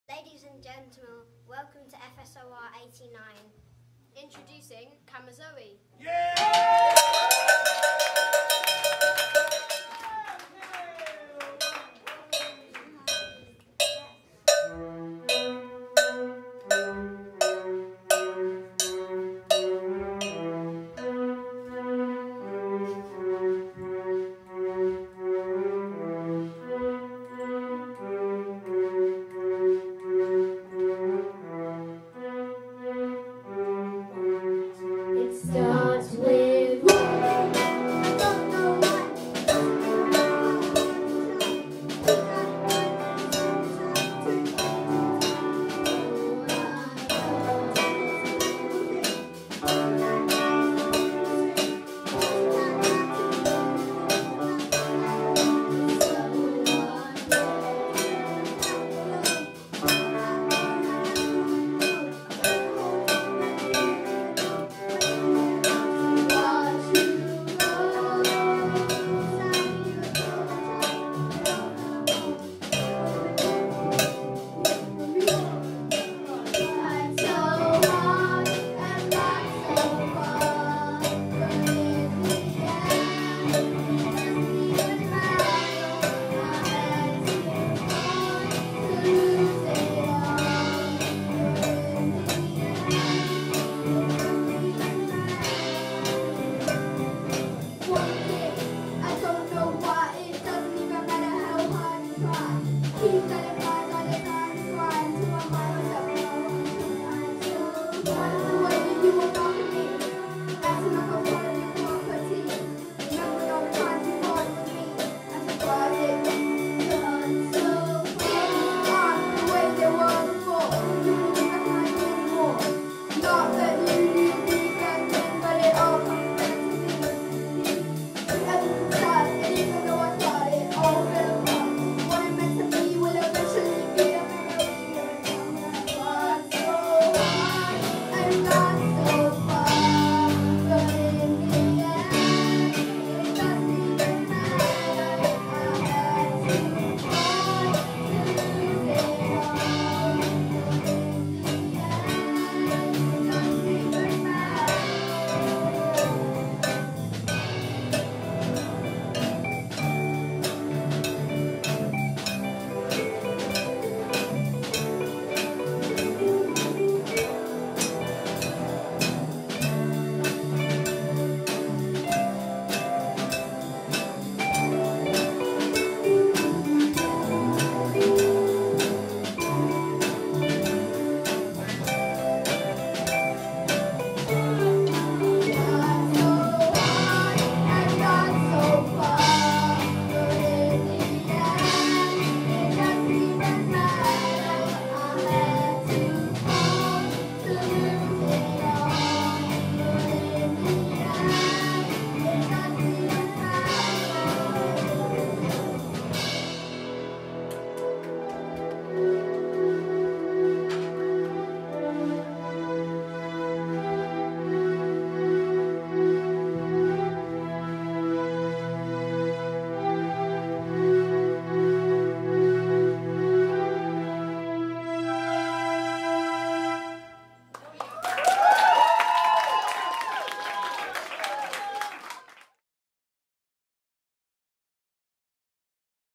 FSOR 89 July 2019 MiniGig: Kamazoe
Felix's School of Rock, London's premiere rock school presents FSOR 89 MiniGig: Kamazoe performing live at their MiniGig (dress rehearsal) with Matthew and Lila on Wednesday July 24th 2019.